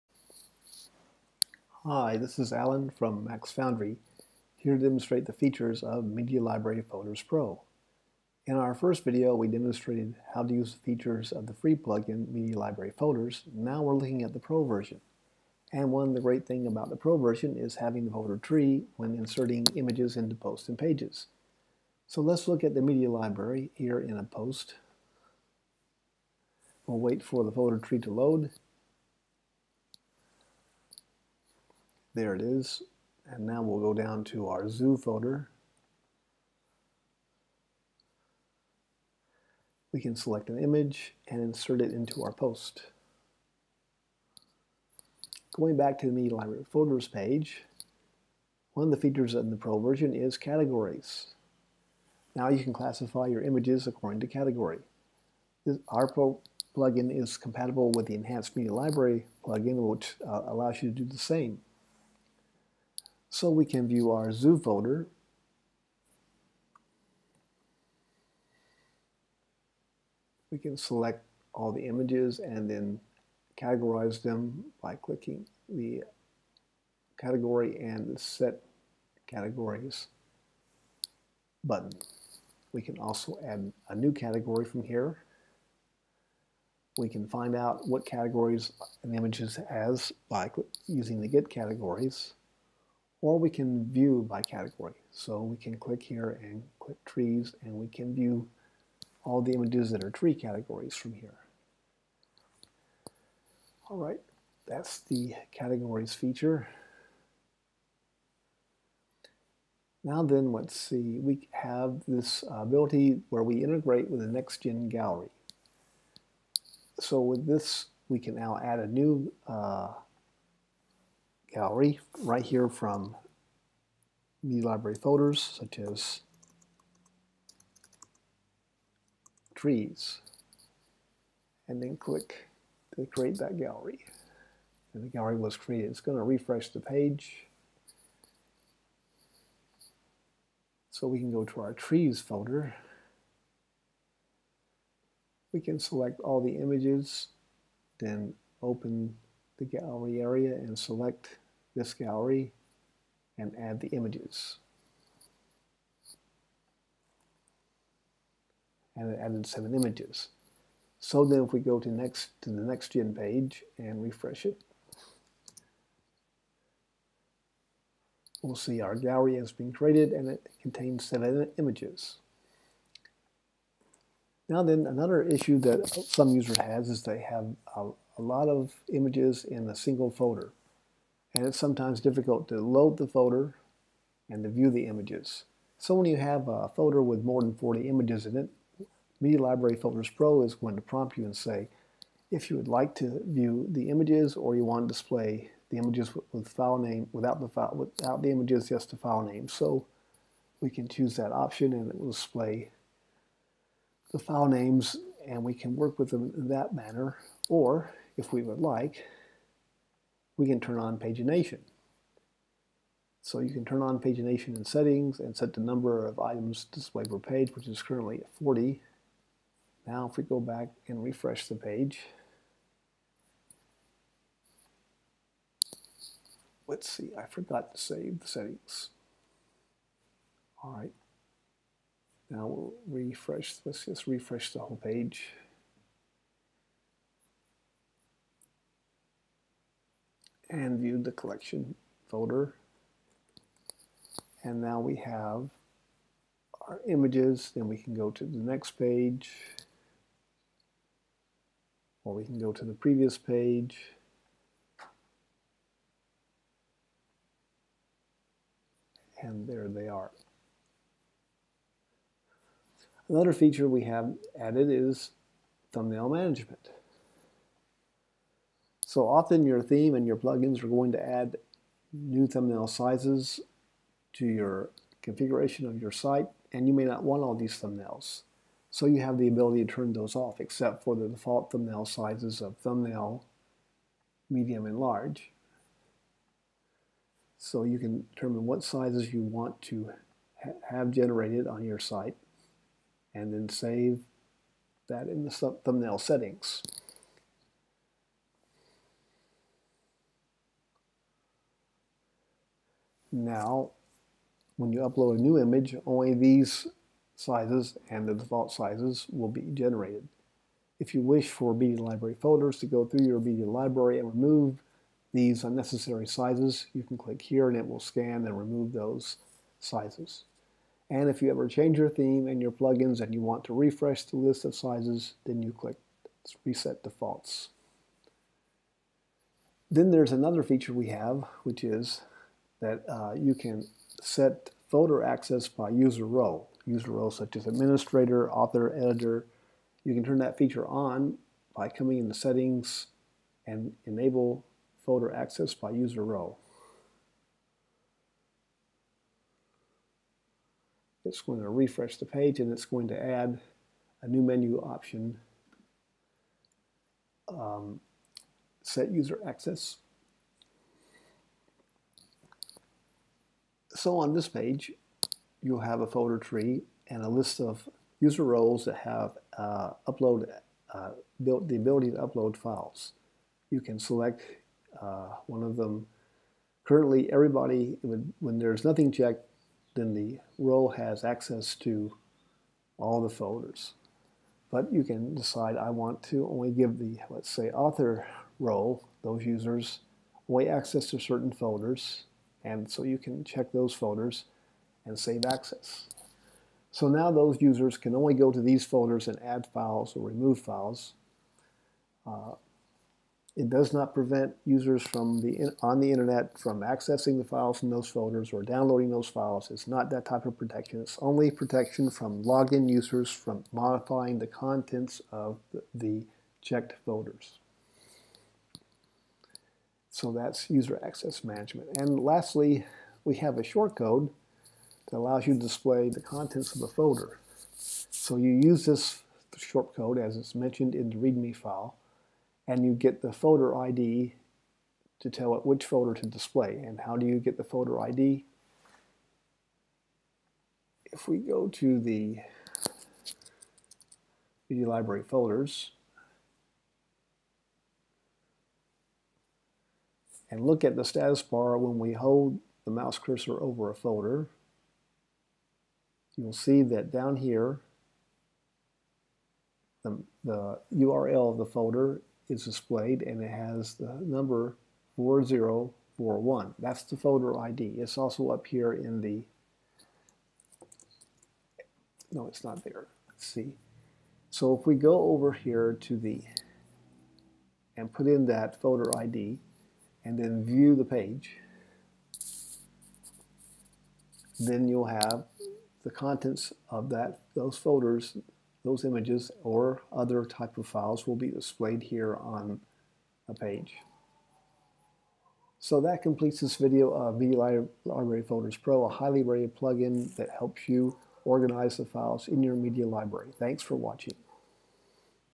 Media Library Folders Pro for Wordpress
A demonstration of the features of the Media Library Folders Pro for Wordpress plugin.